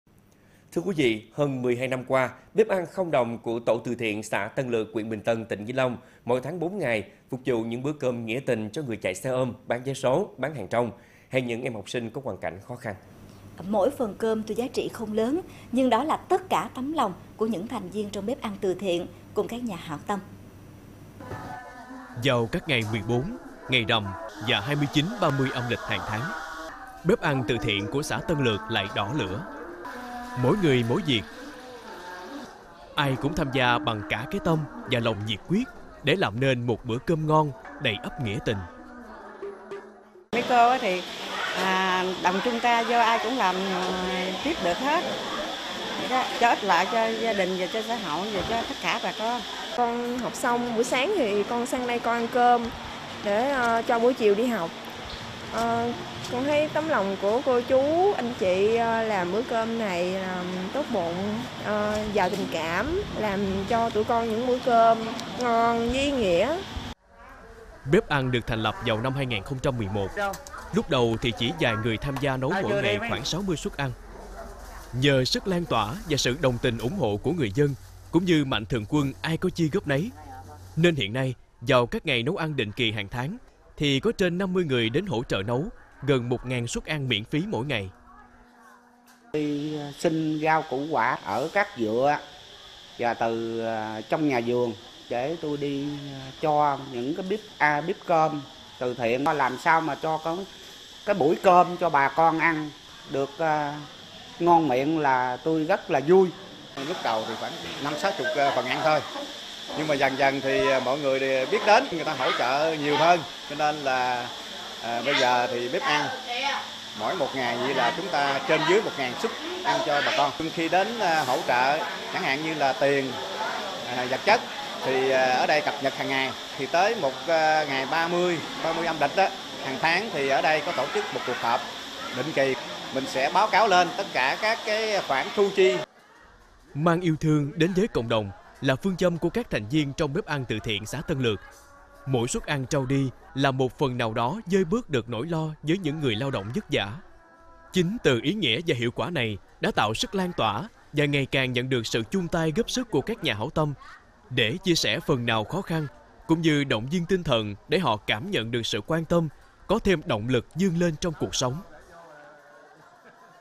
Chuyện tử tế: "Bếp ăn 0 đồng" Tấm lòng thiện nguyện
Hơn 12 năm qua, “Bếp ăn 0 đồng” của tổ từ thiện xã Tân Lược, huyện Bình Tân, tỉnh Vĩnh Long mỗi tháng 4 ngày phục vụ những bữa cơm nghĩa tình cho người chạy xe ôm, bán vé số, bán hàng rong hay những em học sinh có hoàn cảnh khó khăn…Mỗi phần cơm tuy giá trị không lớn nhưng đó là tất cả tấm lòng của những thành viên trong bếp ăn từ thiện, cùng các nhà hảo tâm.